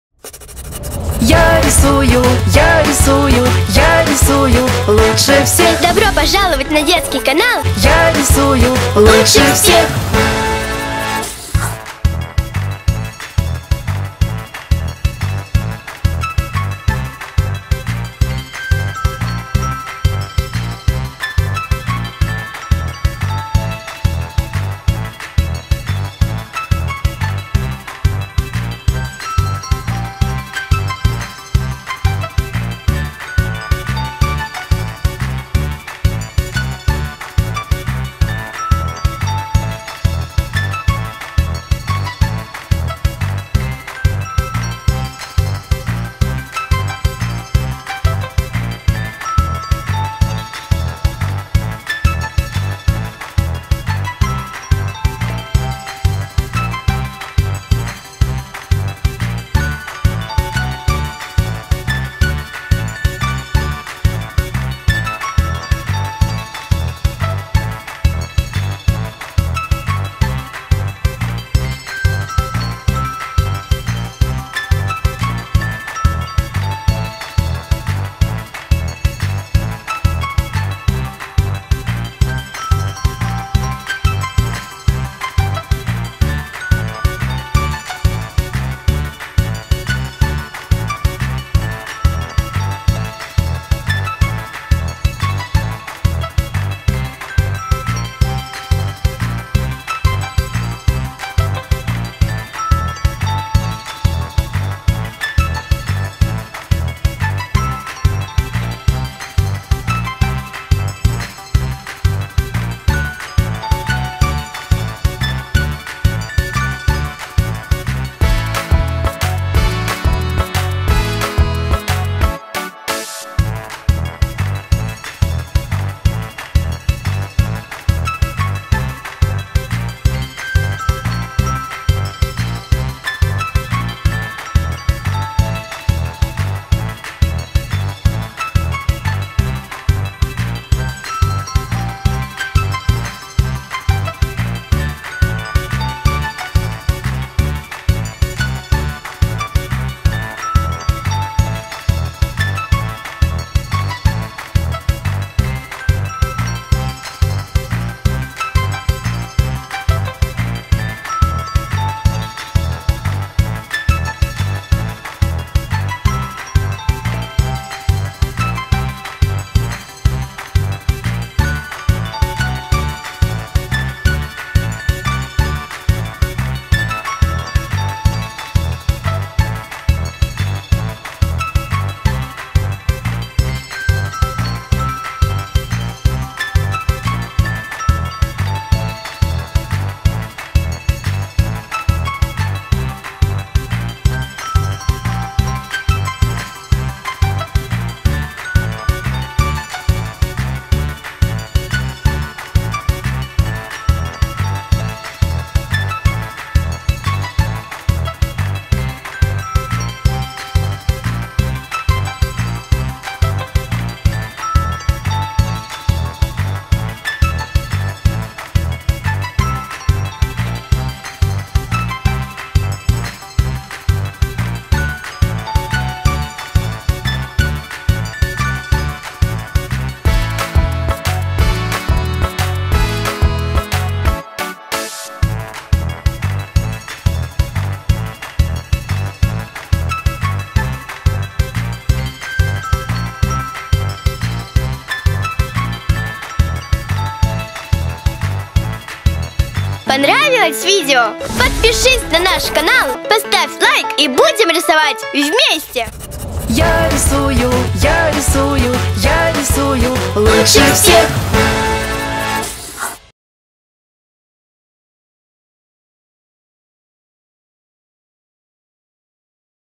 Уроки Рисования Для Детей
Бесплатные уроки рисования для детей и родителей
Сегодня у нас ТУПИК)
Нравится?)
Новые рисунки - каждый день)
Free drawing lessons for children
Today we have a puffin)